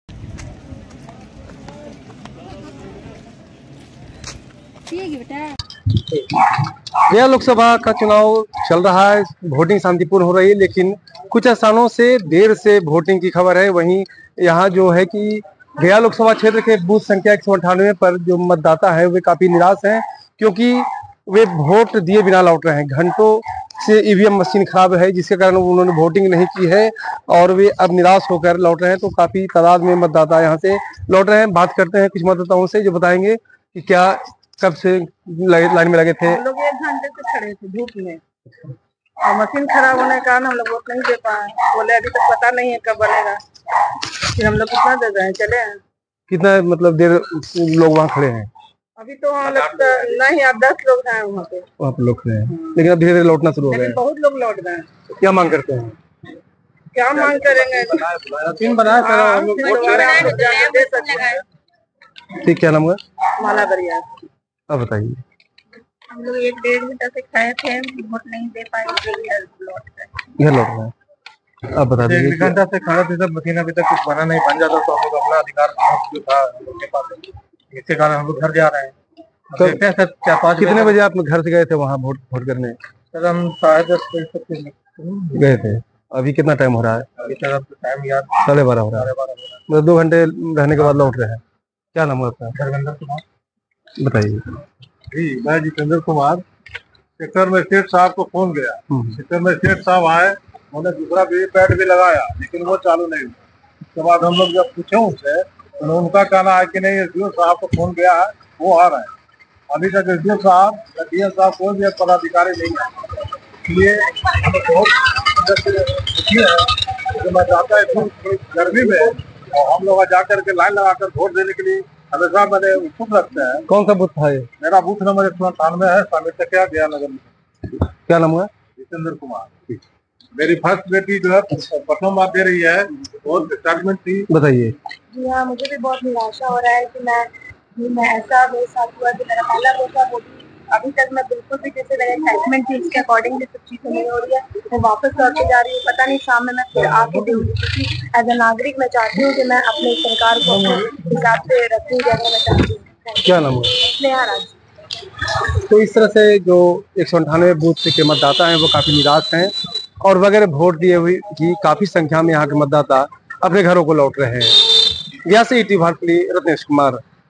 Voting Disrupted In Gaya गया में EVM खराब, डेढ़ घंटे तक बाधित रहा मतदान, परेशान रहे मतदाता
Voting Disrupted In Gaya : लोकसभा चुनाव 2024 के पहले चरण की वोटिंग करायी जा रही है. बिहार के चार लोकसभा में वोटिंग हो रही है. गया में ईवीएम मशीन में खराबी के कारण डेढ़ घंटे मतदान बाधित रहा.

About ETV Bharat Bihar:-

You Can follow ETV Bharat Bihar ( ईटीवी भारत बिहार ) On :-